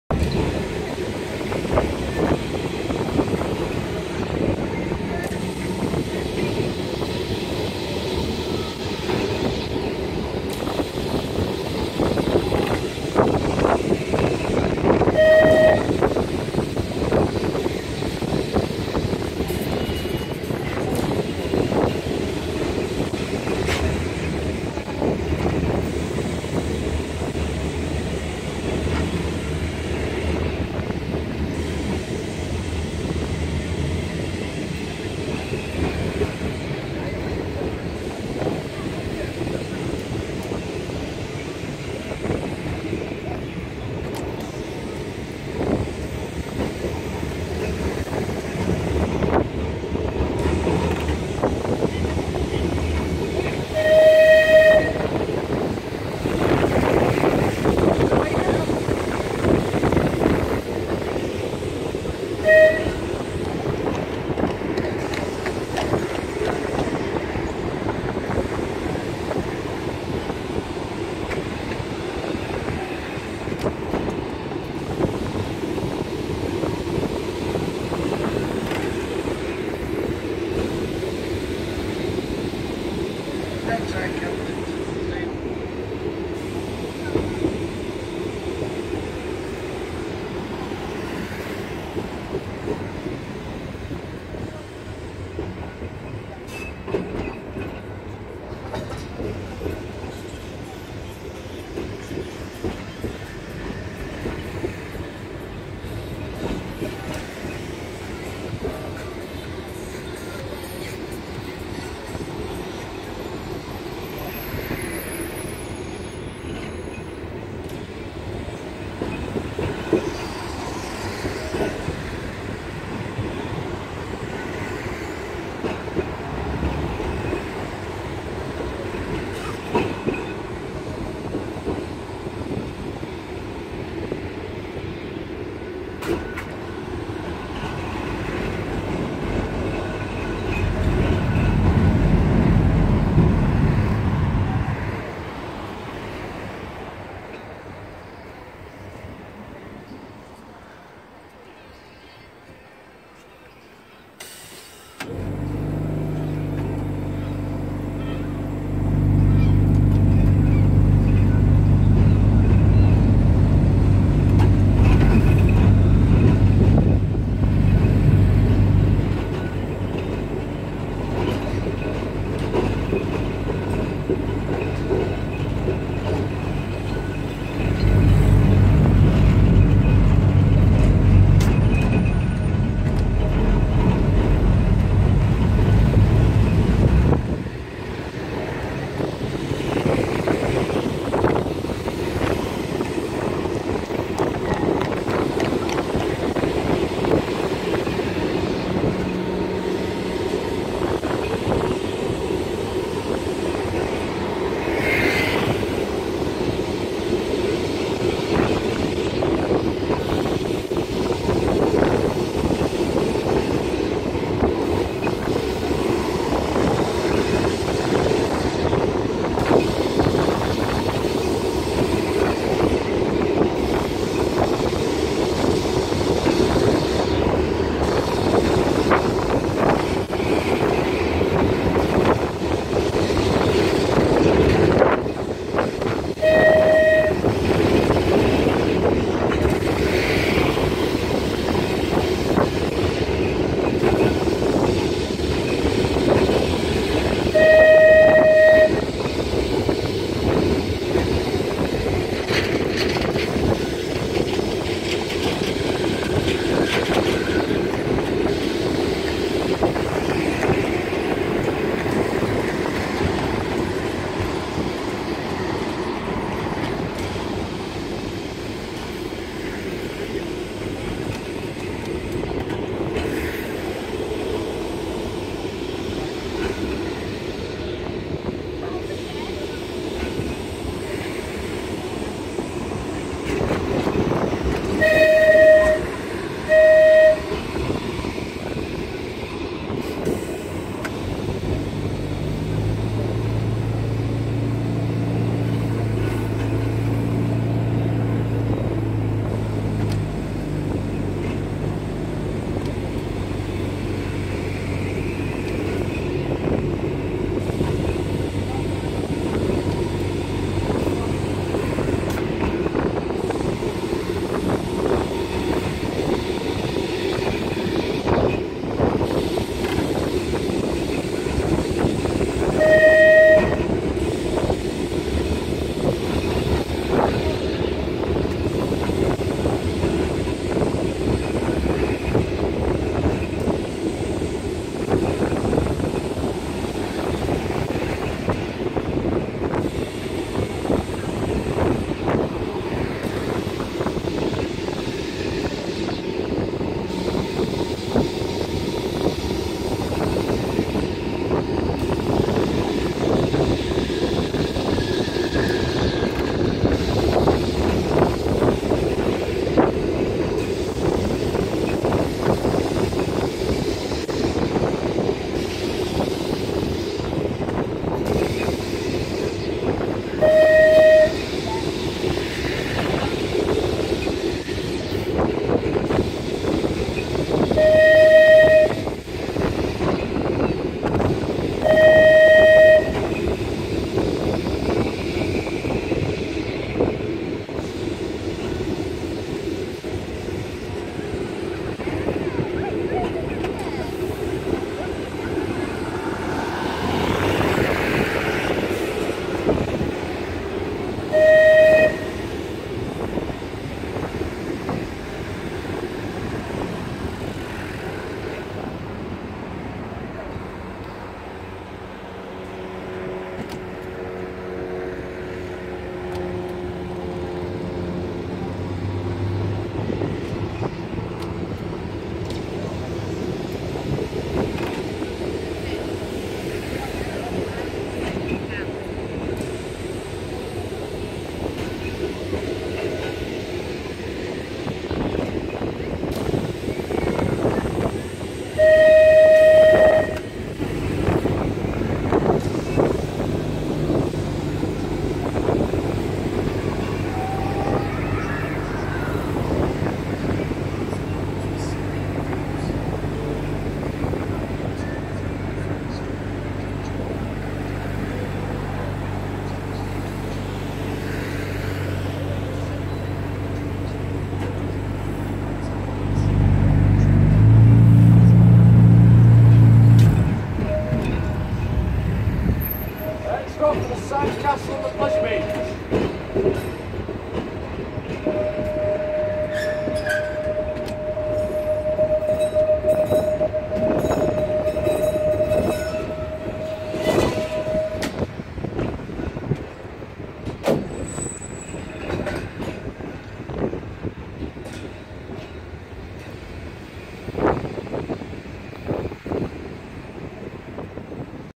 (HD) Blackpool Heritage Tram Boat 227 ride to Pleasure Beach 14/7/2016
Video footage of my first ever ride on Blackpool Heritage Tram Boat 227 from North Pier/Tower to Pleasure Beach, I hoped to ride on Balloon 723 back to North Pier but that wasn't going out for another 30 mins so I jumped back on boat 227 back to north pier instead

I quite like the heritage trams and I'm happy that I have been on one and plan to ride some more at the Tram Sunday event this weekend in Blackpool